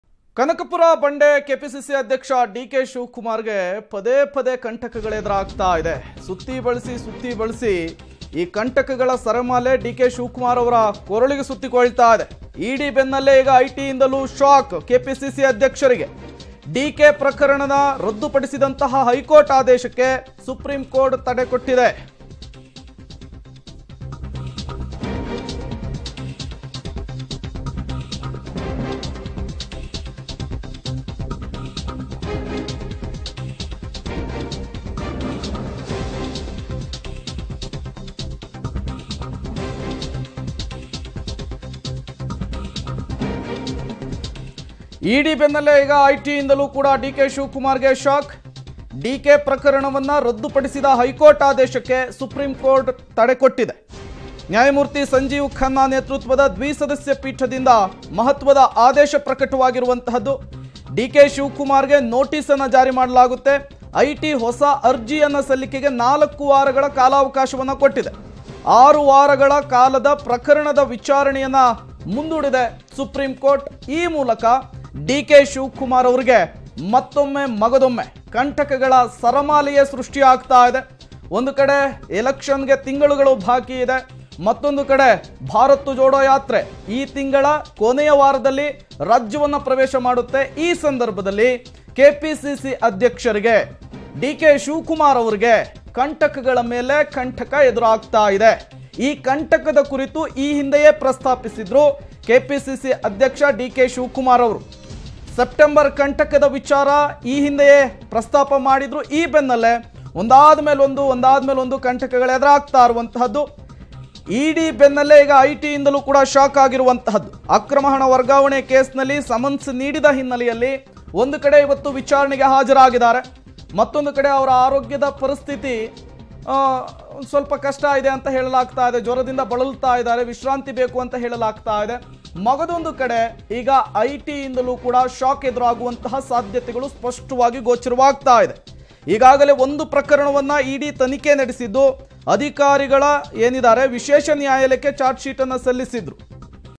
ಡಿಕೆಶಿಗೆ ಇ.ಡಿ. ಬೆನ್ನಲ್ಲೇ ಐಟಿಯಿಂದಲೂ ಶಾಕ್ | Enforcement Directorate | IT | Public TV
ಎಲೆಕ್ಷನ್ ಹೊತ್ತಿನಲ್ಲಿ ಡಿಕೆಶಿಗೆ ಶಾಕ್ ಮೇಲೆ ಶಾಕ್..!
ಇ.ಡಿ ಬೆನ್ನಲ್ಲೇ  ಈಗ ಐಟಿಯಿಂದಲೂ ಶಾಕ್
ಡಿ.ಕೆ ಪ್ರಕರಣ ರದ್ದು ಪಡಿಸಿದ್ದ ಹೈಕೋರ್ಟ್ ಆದೇಶಕ್ಕೆ ಸುಪ್ರೀಂಕೋರ್ಟ್ ತಡೆ
ನ್ಯಾ. ಸಂಜೀವ್ ಖನ್ನಾ ನೇತೃತ್ವದ ದ್ವಿ ಸದಸ್ಯ ಪೀಠದಿಂದ ತಡೆ
ಡಿಕೆಶಿಗೆ ನೊಟೀಸ್ ಜಾರಿ,  ಐಟಿ ಹೊಸ ಅರ್ಜಿ ಸಲ್ಲಿಕೆಗೆ 4 ವಾರ ಅವಕಾಶ
6 ವಾರಗಳ ಕಾಲ ಪ್ರಕರಣದ ವಿಚಾರಣೆ ಮುಂದೂಡಿದ ಸುಪ್ರೀಂಕೋರ್ಟ್